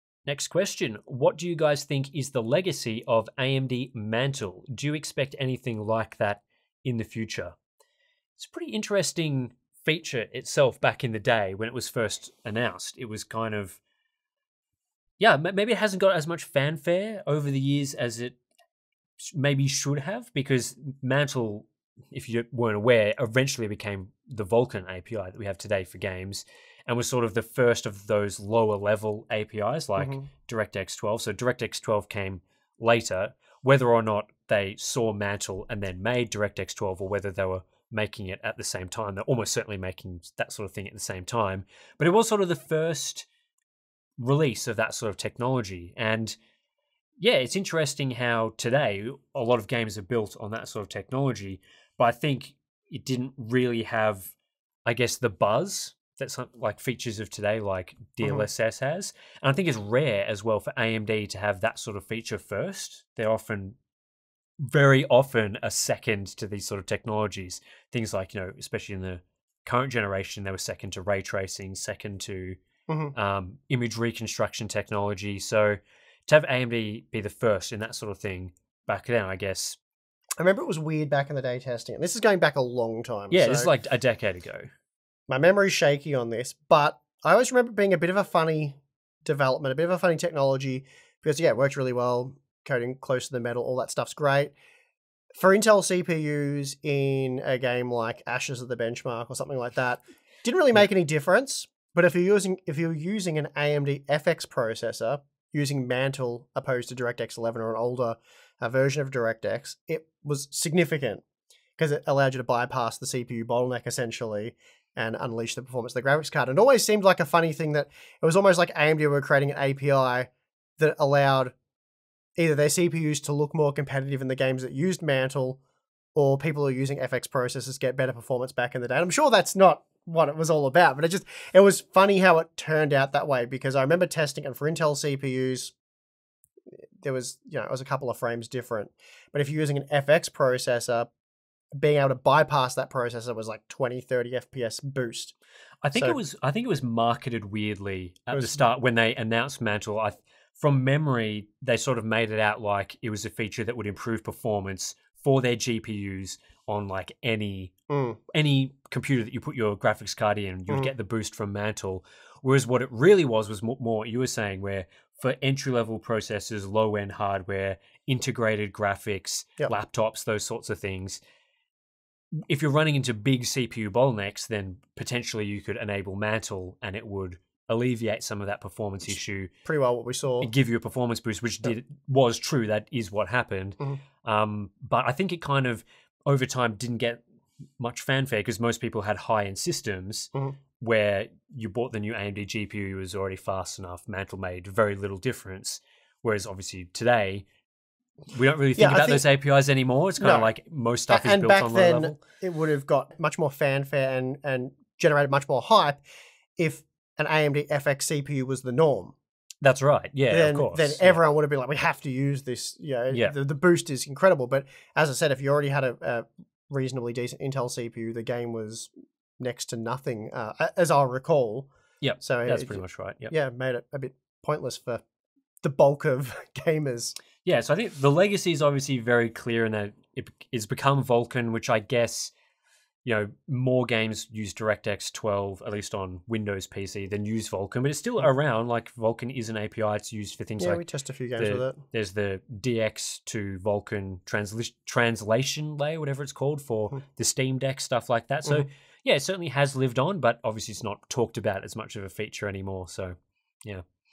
What does the future hold for AMD Mantle?
Today Tim and Steve discuss AMD Mantle and what could be on the cards for future technologies in the space.